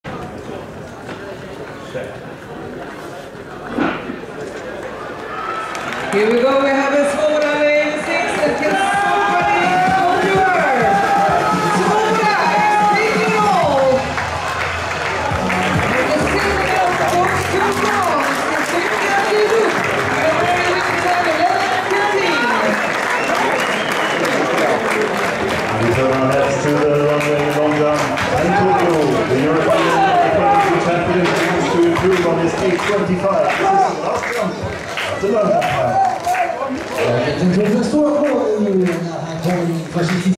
V 100m Finale
Een erg sterke nieuwe Europese Kampioene; de Poolse Ewa Svodoba wint deze finale in een snelle 11.15 (+0.6).   Het zilver wordt gewonnen door de Francaise Cynthia Luduc in 11.40 en Lisa Nippgen uit Duitsland wint de bronzen medaille in 11.45.